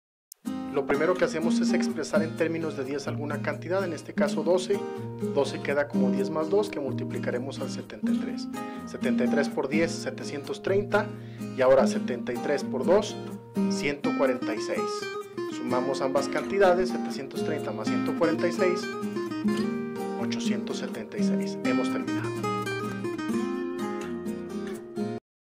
Método simple para multiplicar cantidades grandes (Ejemplo I)
En la presente cápsula ofrecemos un método para multiplicar cantidades grandes. El procedimiento consiste en expresar o descomponer una de las dos cantidades a multiplicar: en términos de 10, 100, 1000, etc., según sea necesario, y multiplicar el otro número por esta descomposición, al final sumar los resultados parciales.